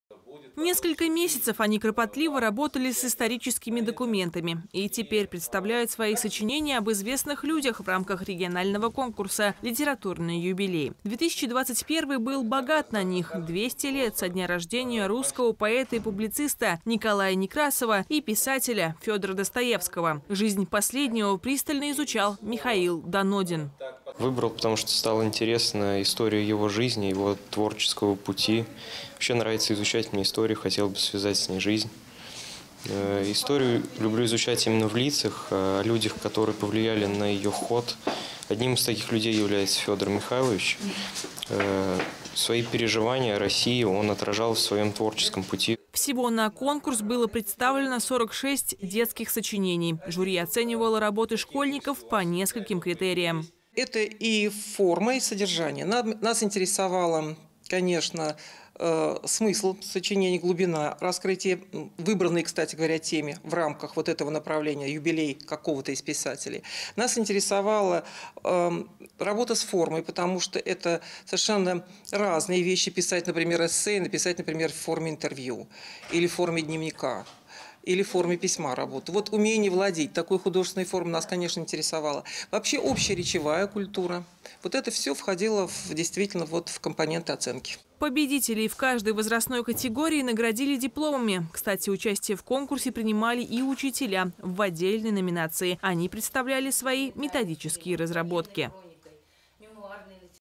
В Пензе подвели итоги регионального конкурса сочинений «Литературные юбилеи»
Он был возрожден в этом году и прошел в новом формате. В нем принимали участие не только школьники, но и преподаватели. Последние представили свои методические разработки уроков и внеклассных занятий по предложенной теме.